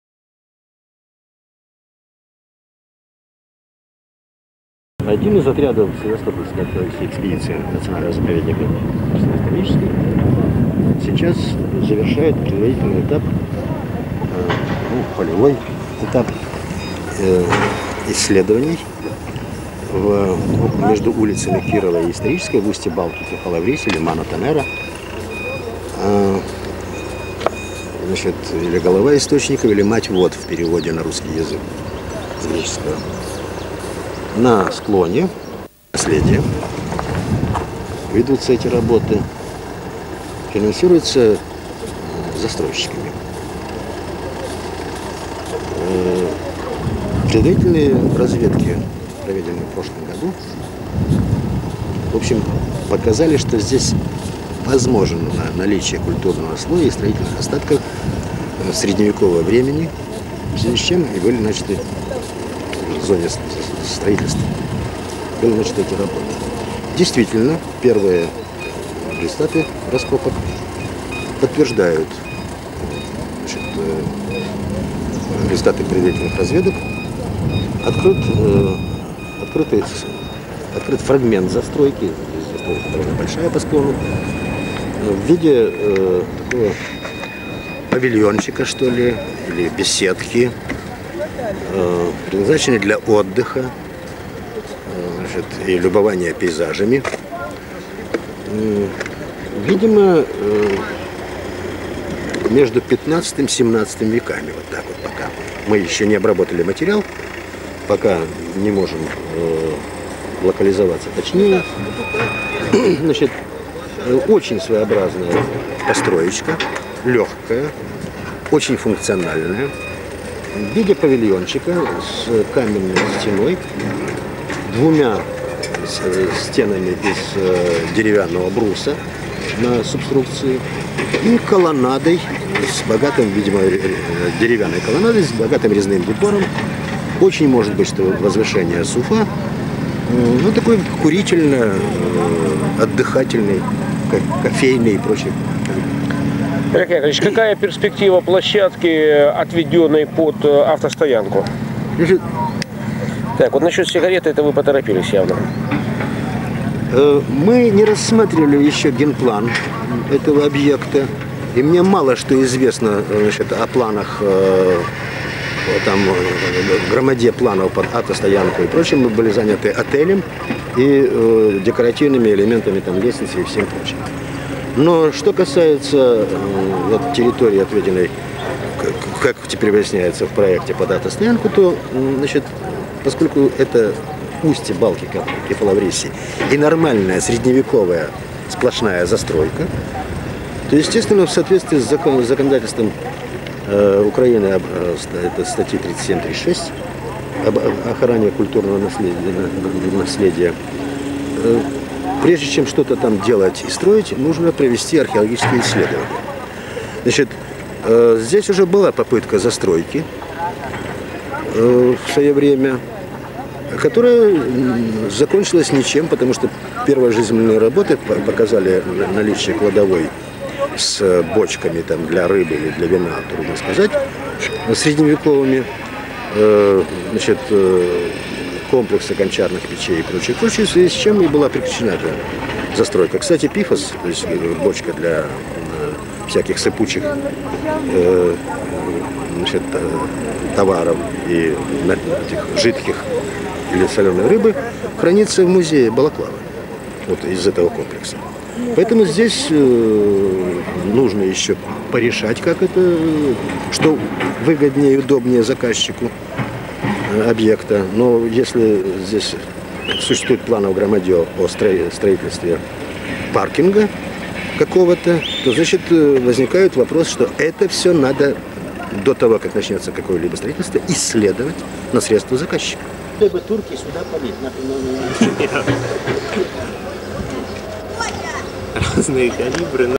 2001 Савеля в Балаклаве
Археолог Олег Яковлевич Савеля знал Балаклаву, как никто другой. Он был уверен, что в ней еще огромное количество загадок. И сам искал ответы.